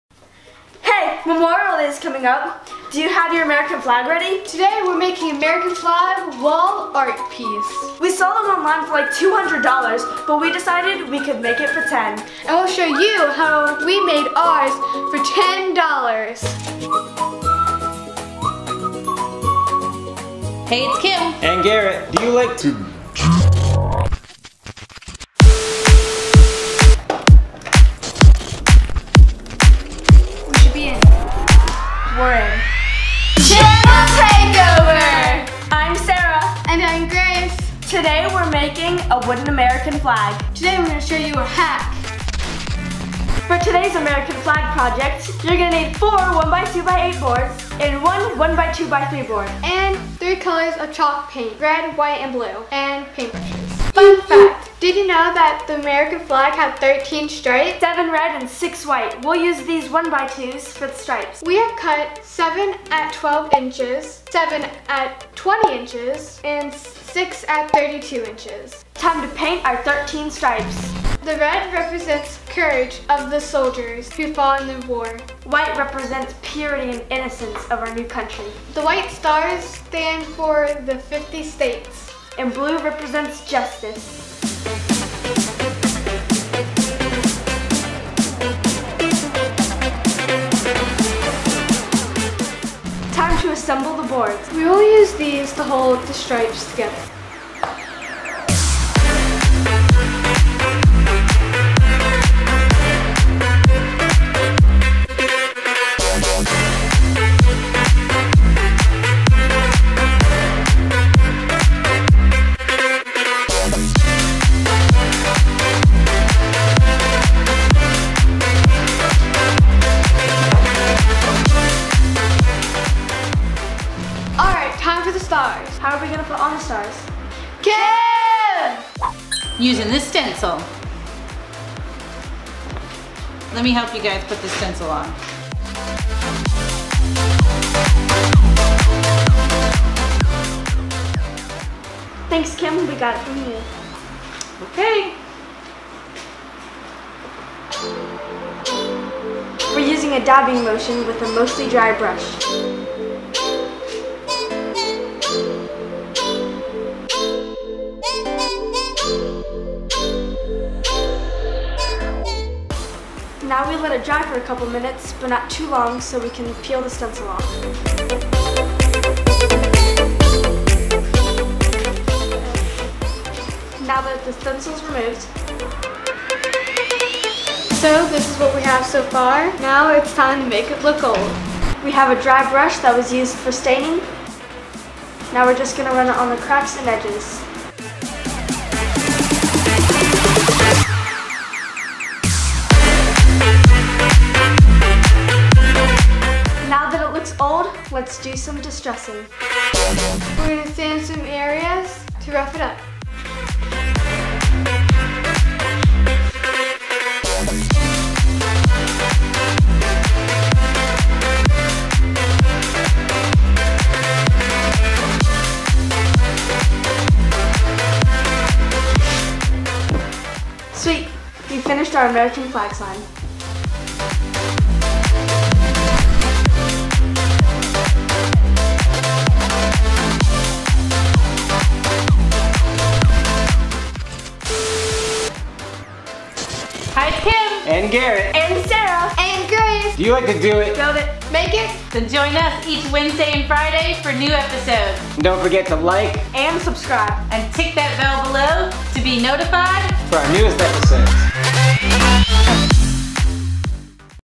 How To Make a Rustic American Flag
This week the girls took over the channel to show you how to make a rustic American Flag Wall Art piece. This was a REALLY easy project.

Material List:
4 - 1X4s @ 8'
1 - 1X2  @ 3'
Red, White and Blue Paint
Star Stencil or just draw them

Cut List:
7 - 12" 1X2s
7 - 20" 1X2s
6 - 32" 1X2s
3 tack strips

Paint the 7 12" boards blue
Paint 4 of the 12" boards red
Paint 3 of the 12" boards white
Paint 3 of the 32" boards red
Paint 3 of the 32" boards white

Place them face down, glue and nail the tack strips to the back.

Flip it over and paint the white stars, we used a stencil.

Run a dry stain brush of the edges and cracks to give it the vintage aged look.  then, run some sand paper over some areas to thin out the paint and let some of the board just barely show through to make it rustic.  Sand and stain until you get the look you want.

Done!  Easy!

We love to Do It Ourselves. You love to Do It Yourself.  Let's Do It Together.